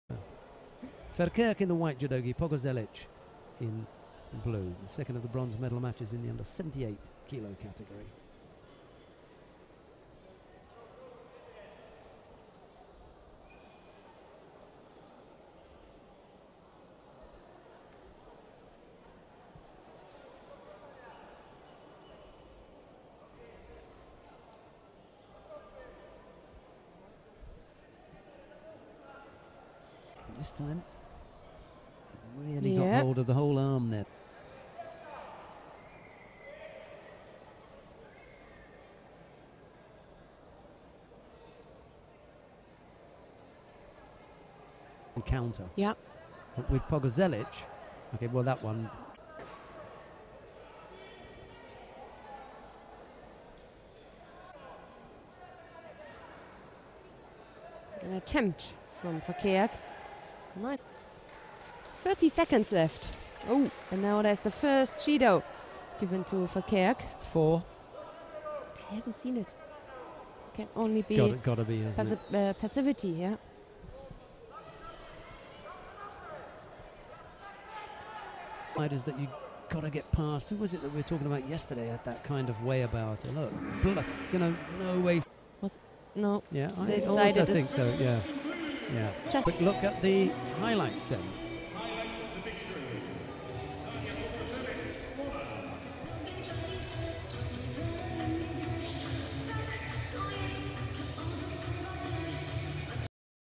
Judo 2015 Grand Slam Baku: Verkerk (NED) - Pogorzelec (POL) [-78kg] bronze
Marhinde Verkerk (NED) - Daria Pogorzelec (POL) [-78kg] bronze
Judo 2015 Grand Slam Baku